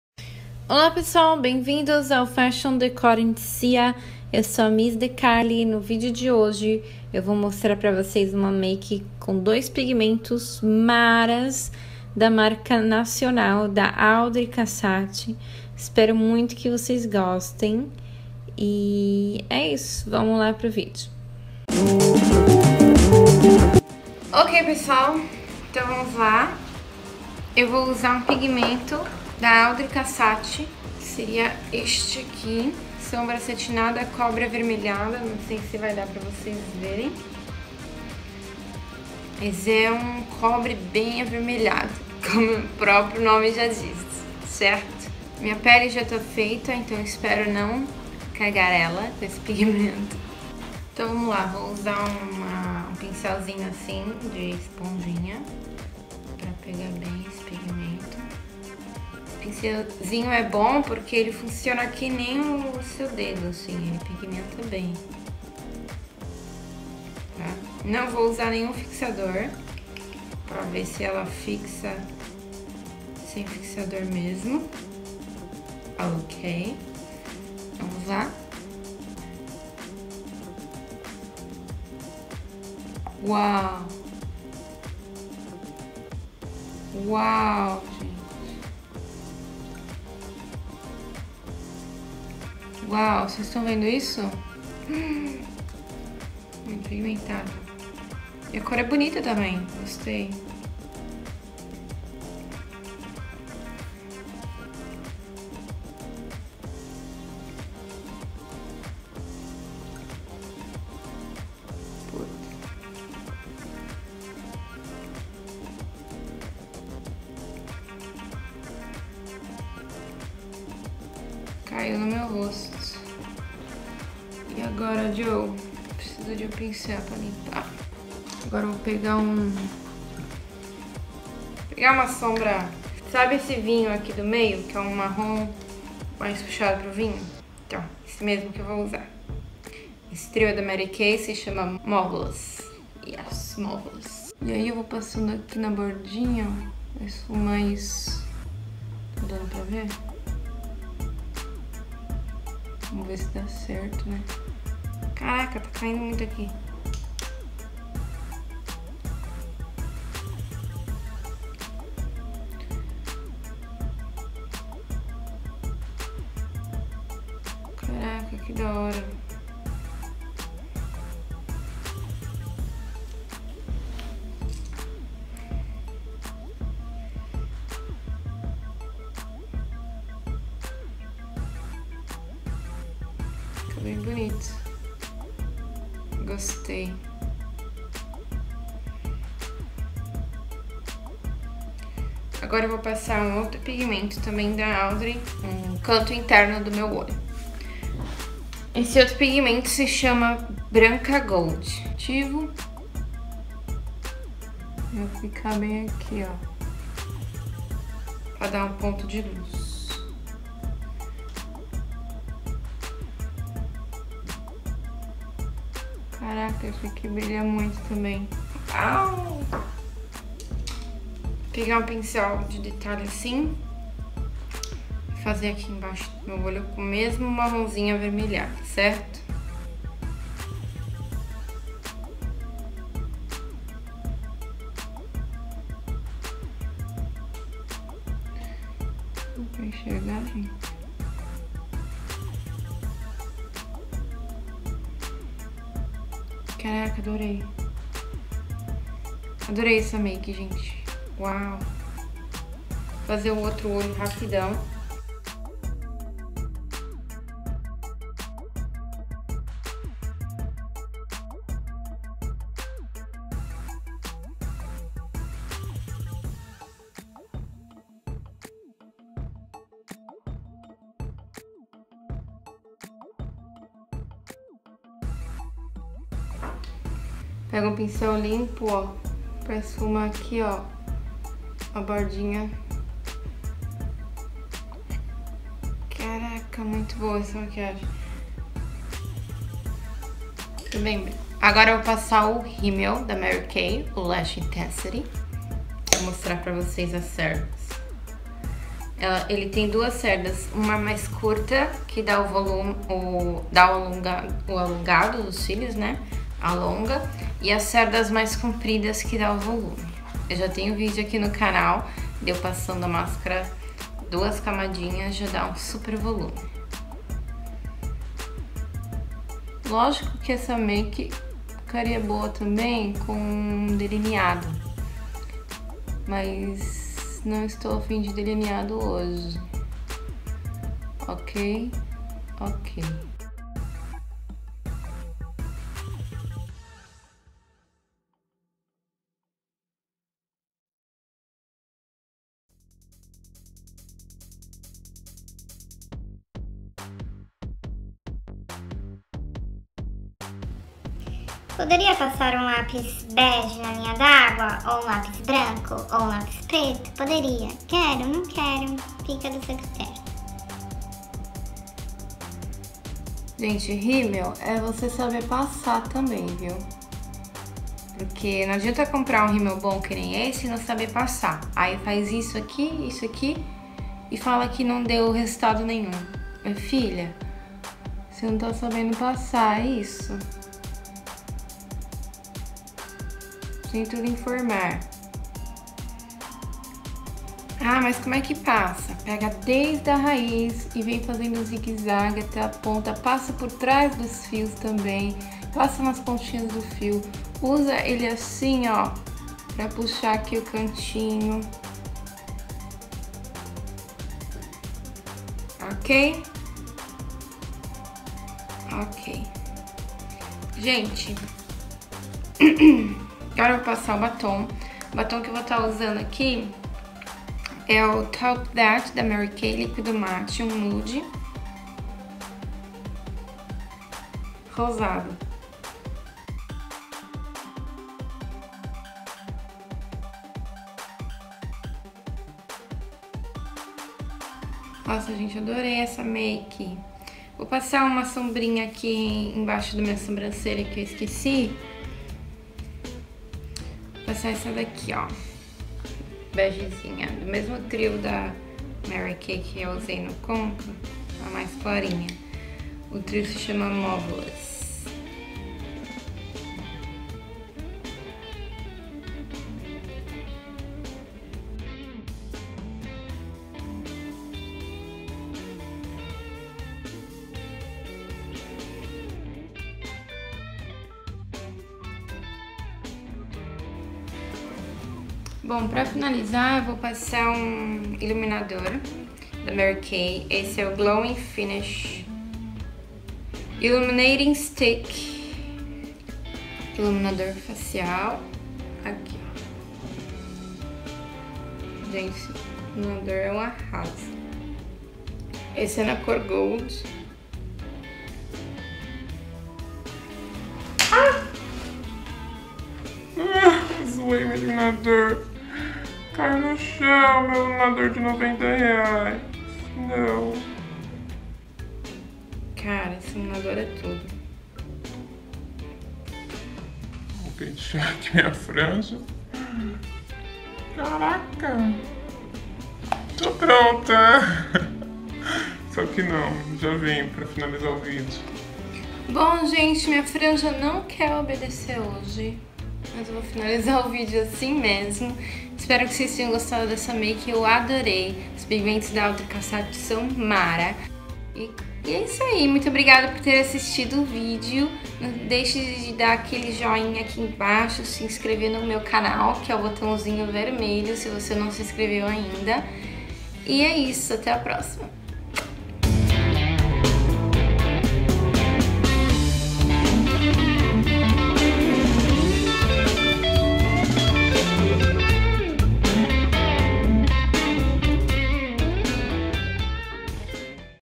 Maquiagem com pigmentos Audrei Casatti
Olá pessoal!!
No vídeo de hoje fiz uma make com dois pigmentos da Audrei Casatti, aameii o resultado!!! Estou apaixonada pelas cores!
Me contem o que acharam!

Pigmentos usados:
121- Sombra acetinada cobre avermelhado
24- Branca ouro

Trio Mary Kay: Mauvelous

xoxo!